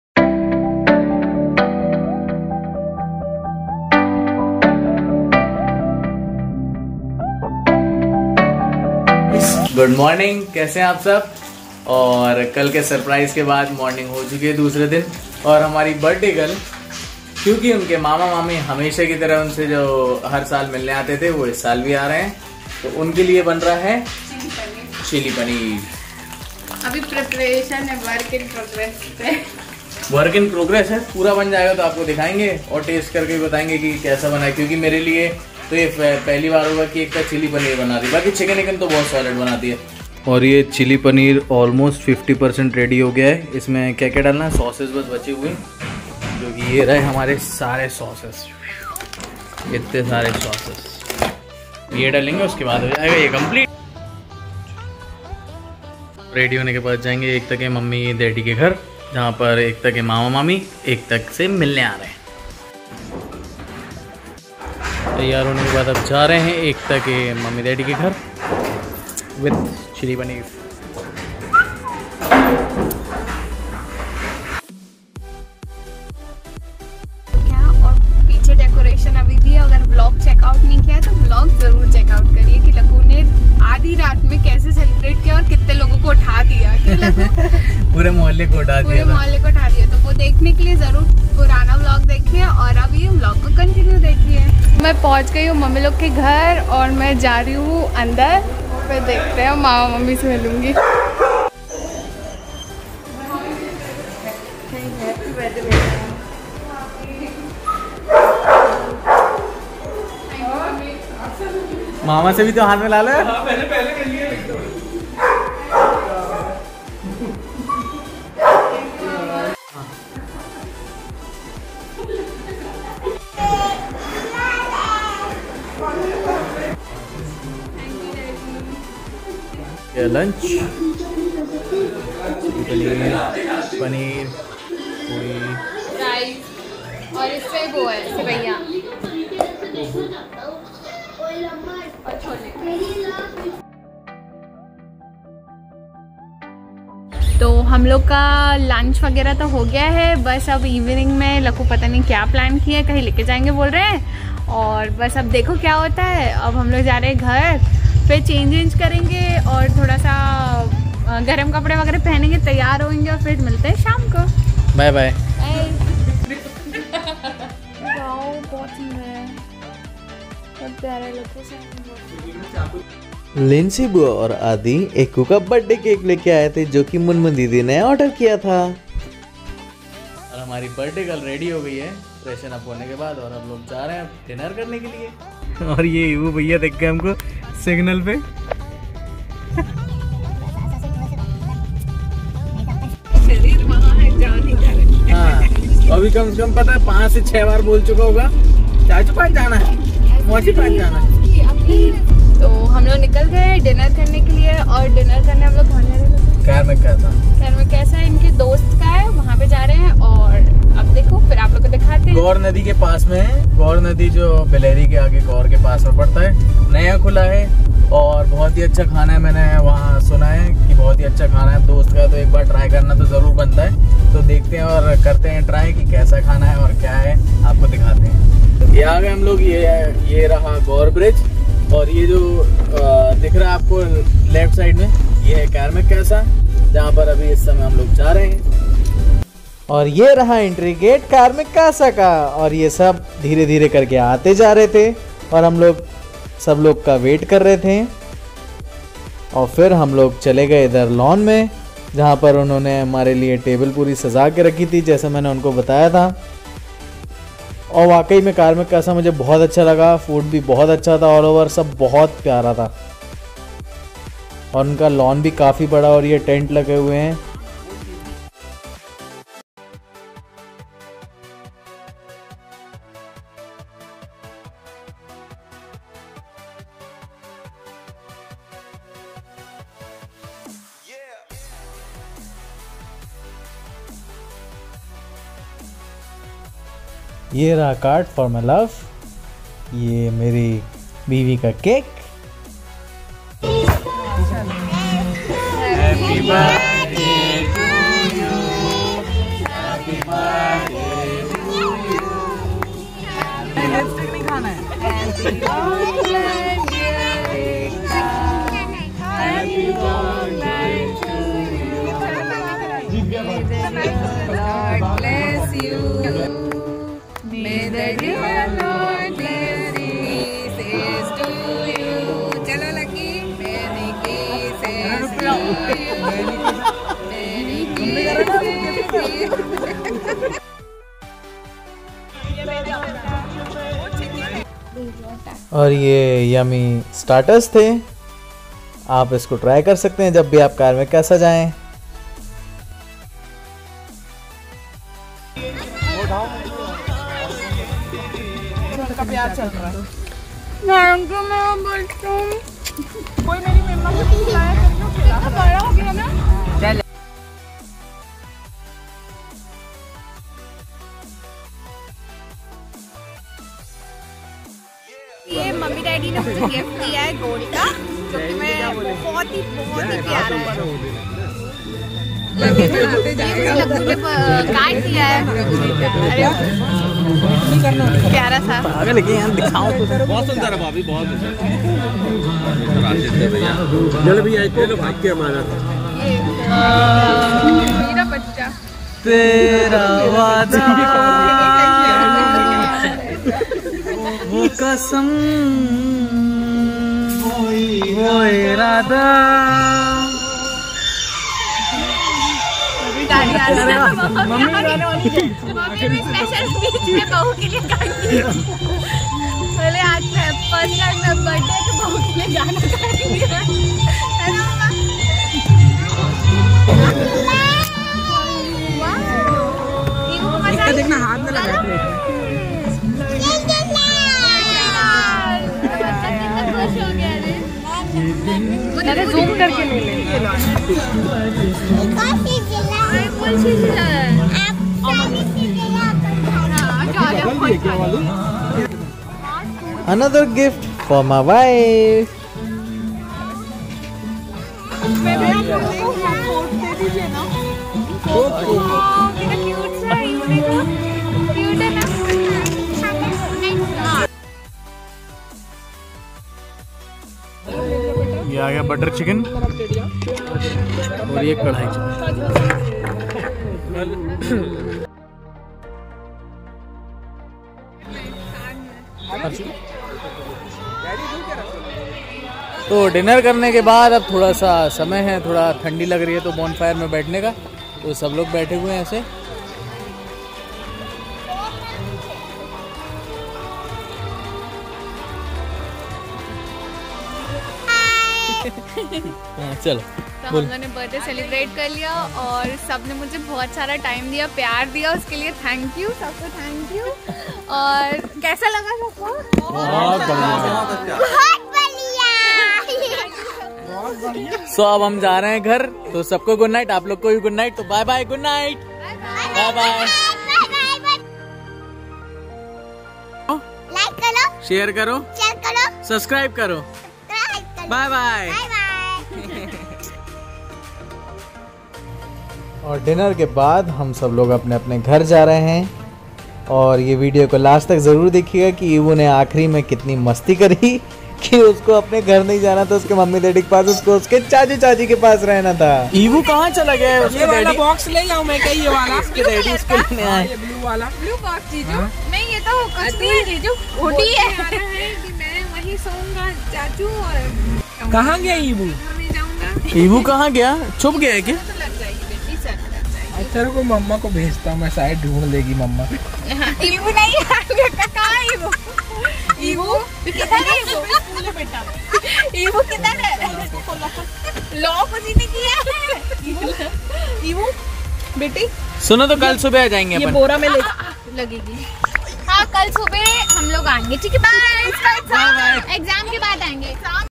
ekku ka birthday celebration part 2 | karmic casa jabalpur | Lucky ki ekku
Thank you for loving our night birthday celebration video... Sorry for upload the part late as we were busy with some personal issue..

love
AE

Link of the night birthday celebration...